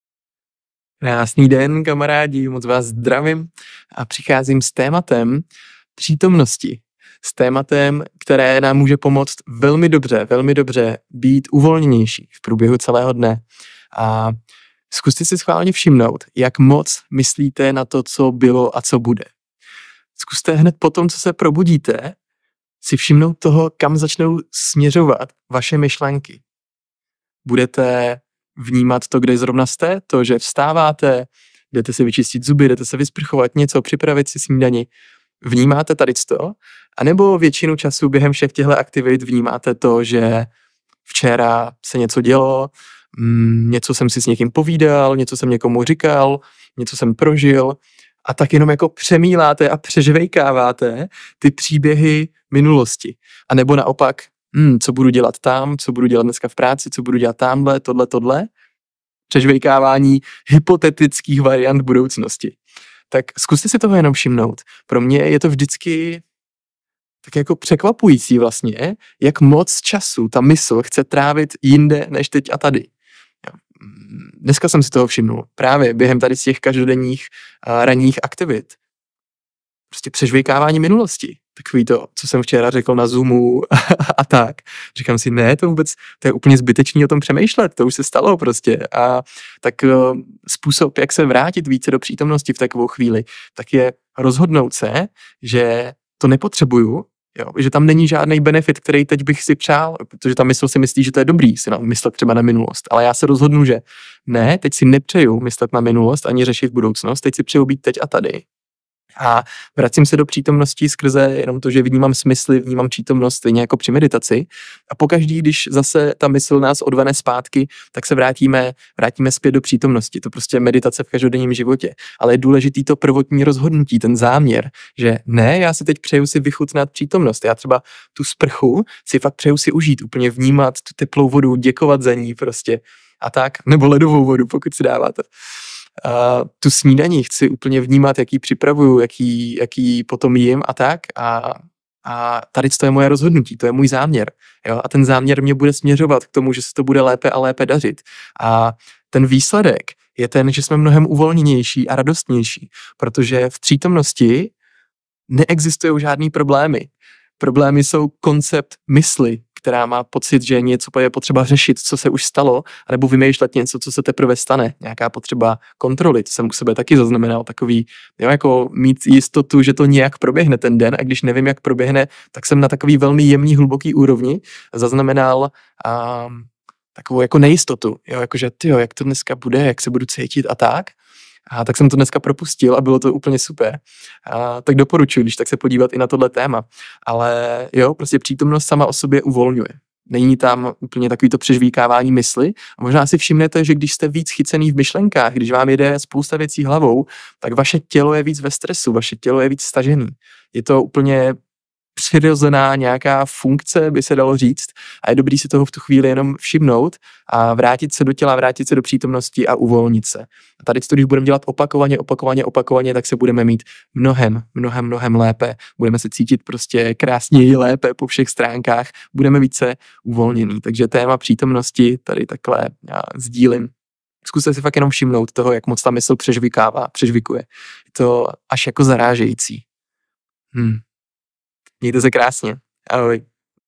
Přítomnost uvolňuje – jste tady a teď?
Kratičká epizoda o tom, jak je uvolňující být více tady a teď. A jak na to!